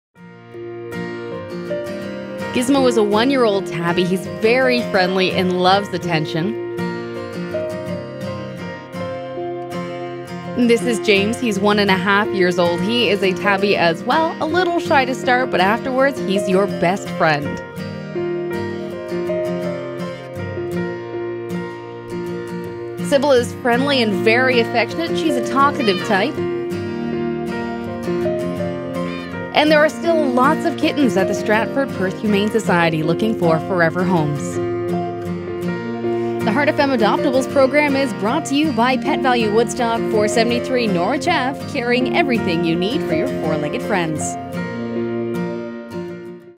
Heart FM Adoptables - September 01
we visit the Stratford-Perth Humane Society to see who's looking for
forever homes